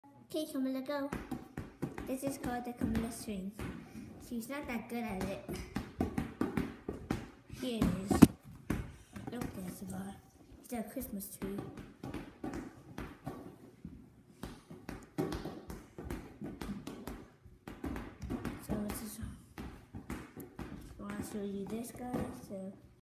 The Camilla swing
My sister made up her own basketball move